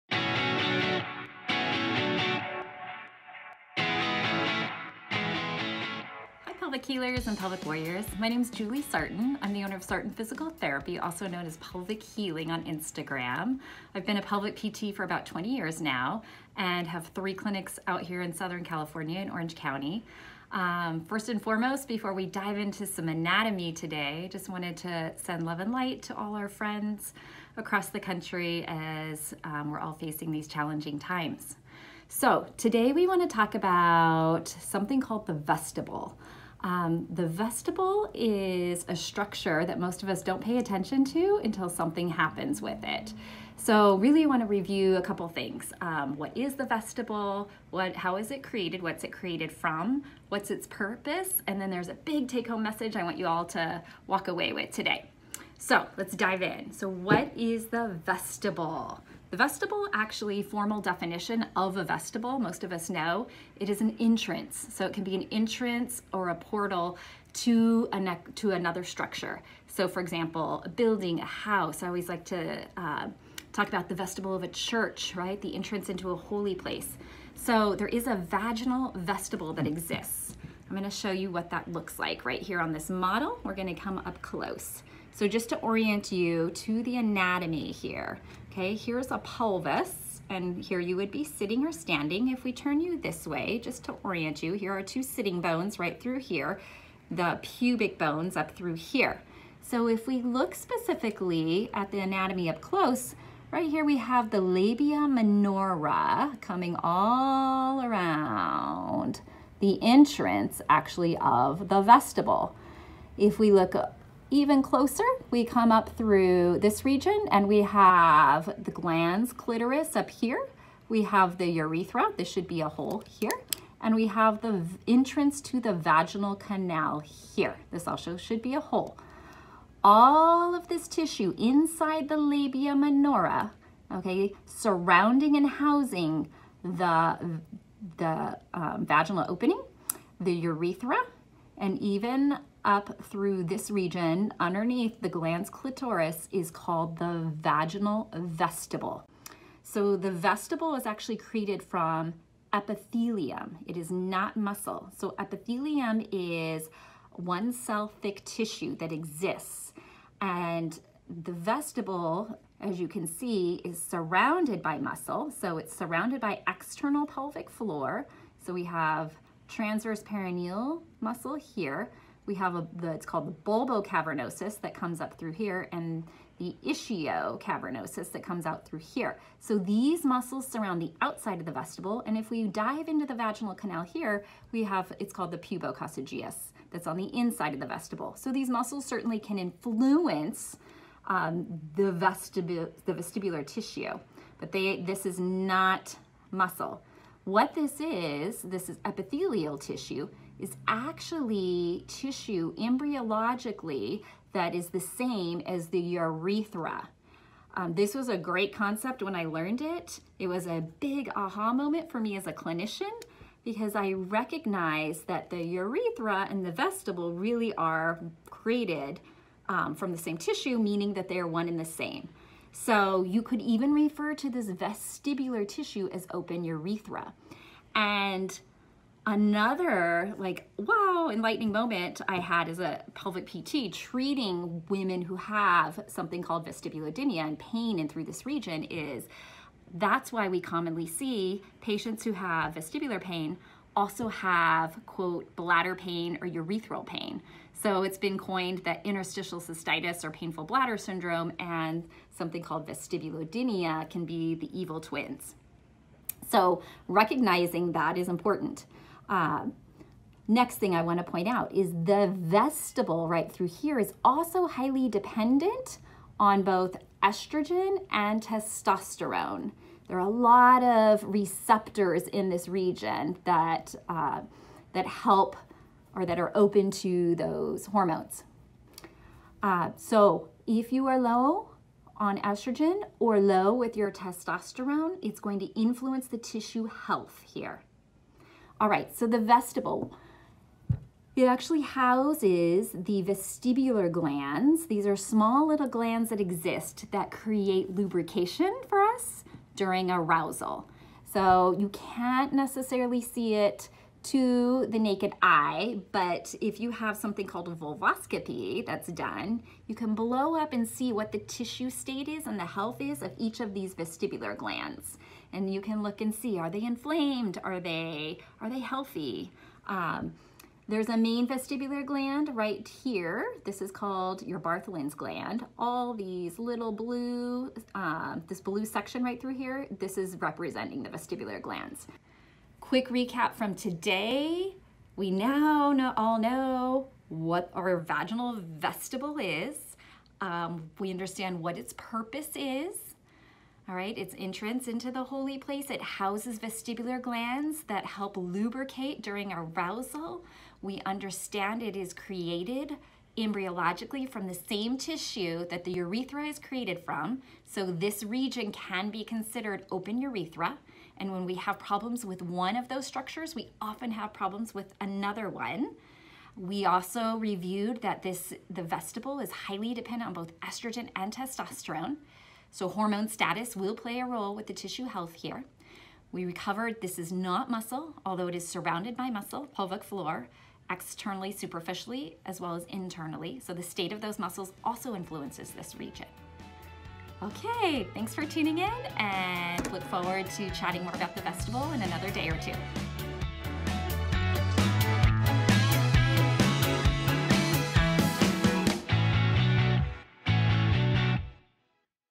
The Vaginal Vestibule | Anatomy & Purpose
Let's Talk About The VAGINAL VESTIBULE!

Dr. Julie Sarton here. ⁣⁣
The vestibule is a structure that most of us don't pay attention to until something happens with it (sounds familiar, right?)⁣⁣
In this video, I'll be covering:⁣⁣
1. What is the vaginal vestibule? (definition & anatomy)⁣⁣
2. What it is created from? (tissue breakdown)⁣⁣
3. My HUGE "aha!" moment (changed my practice as a clinician)⁣⁣
4. What is the purpose of this structure? ⁣⁣
This video also covers why vestibulodynia and interstitialcystitis (painful bladder syndrome) are commonly coined as "evil twins".⁣⁣
1. How we diagnose issues within the vestibule⁣⁣
2. What treatment consists of⁣⁣